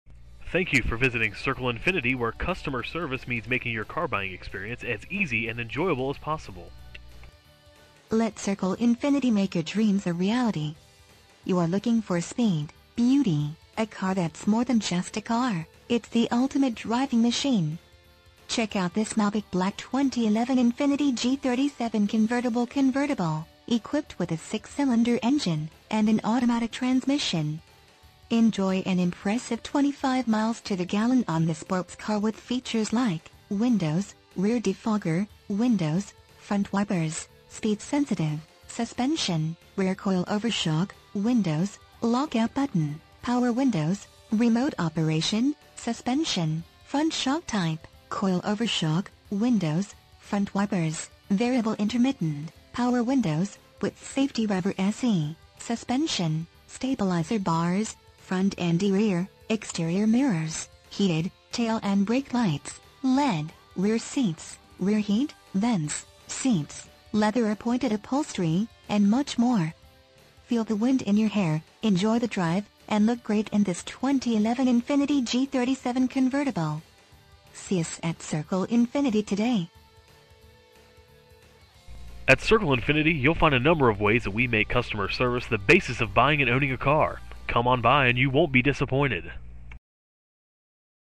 2011 Infiniti G37 Convertible Circle Infiniti West Long Branch, NJ 07764
Infiniti G Convertible Imagine driving this Malbec Black Infiniti G37 Convertible Convertible, equipped with a 6 Cylinder engine and an automatic transmission.
MILES: 21,569
VIN:JN1CV6FE5BM951975

Circle Infiniti
301 Highway 36
West Long Branch, New Jersey 07764